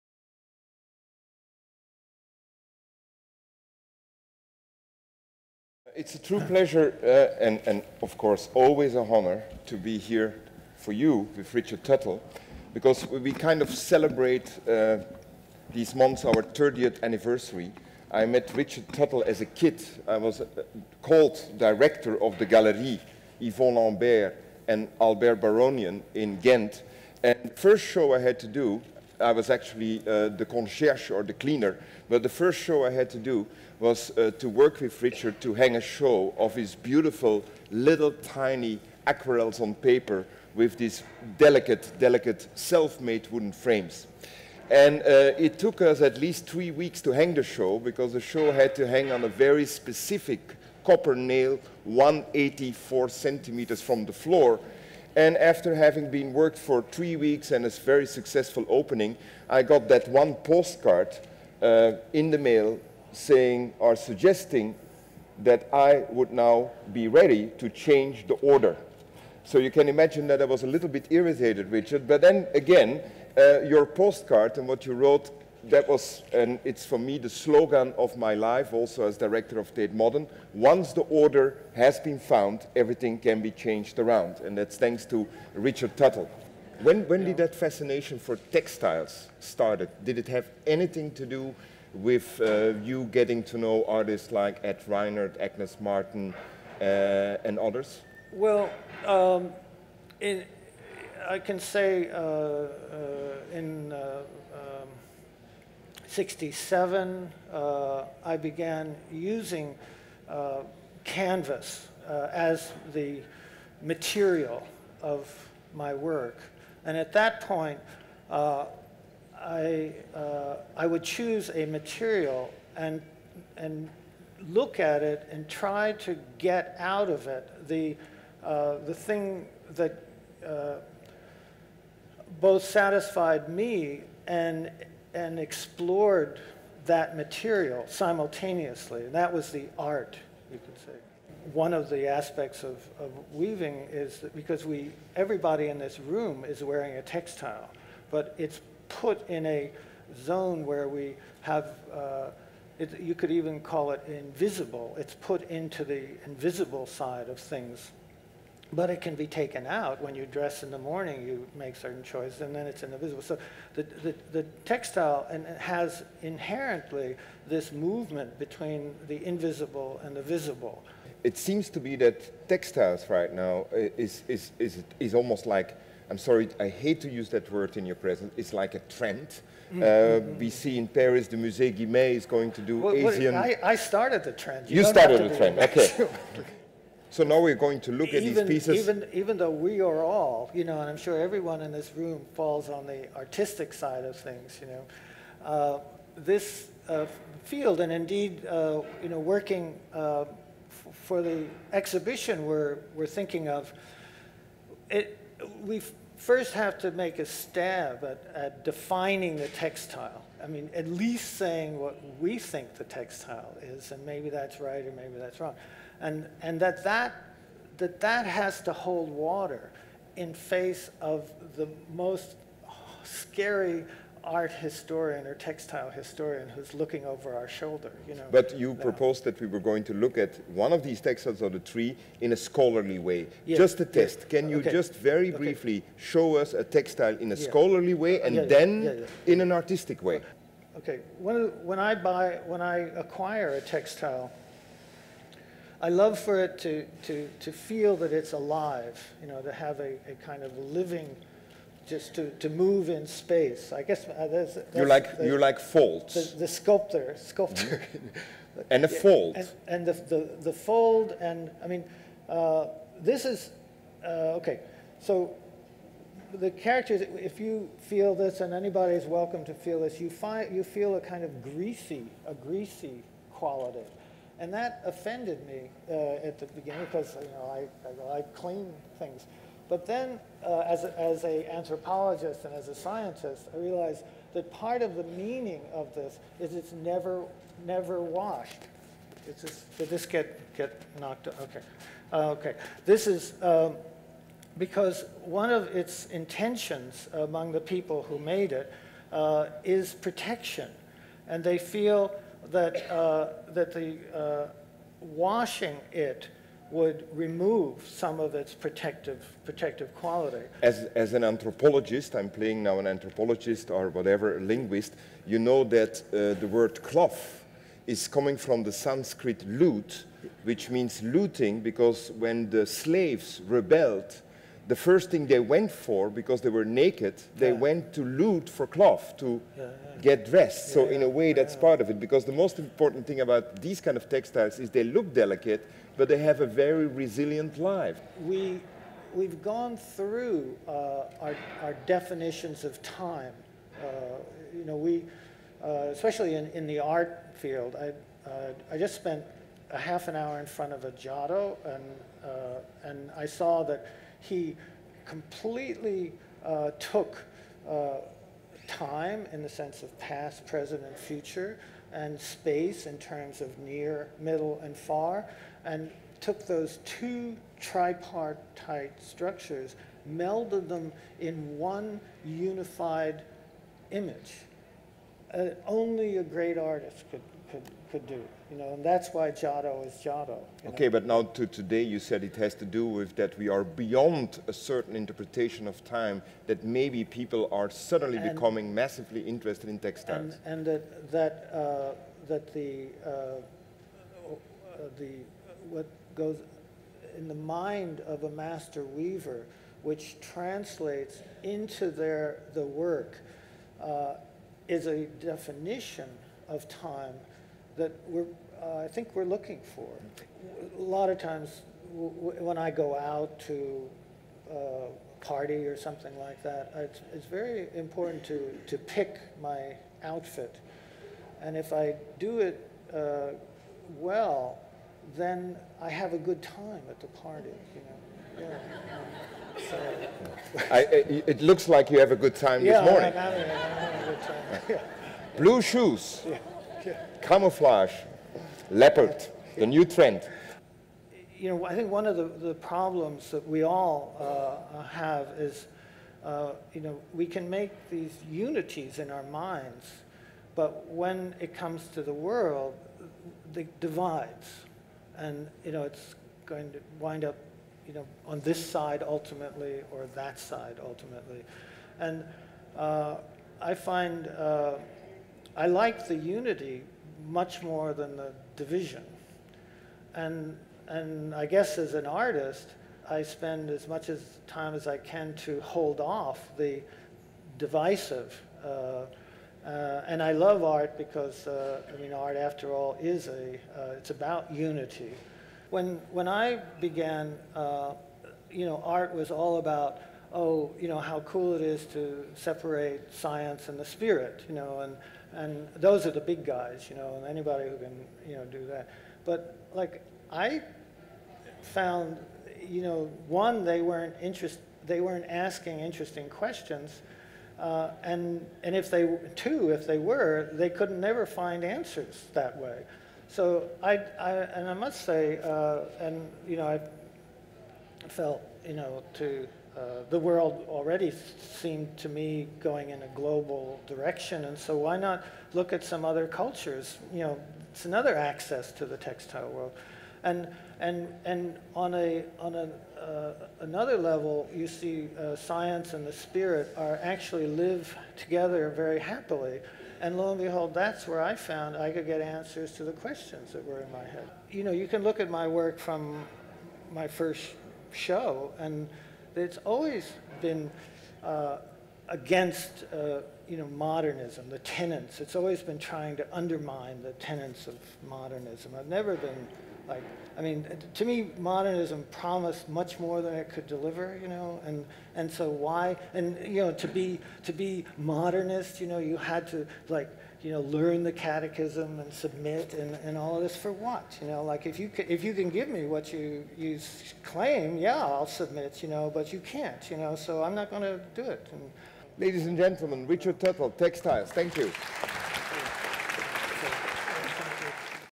Conversations | Premiere | Artist Talk | Richard Tuttle (short version)
Richard Tuttle, Artist, New York/New Mexico
In conversation with Chris Dercon, Director of Tate Modern, London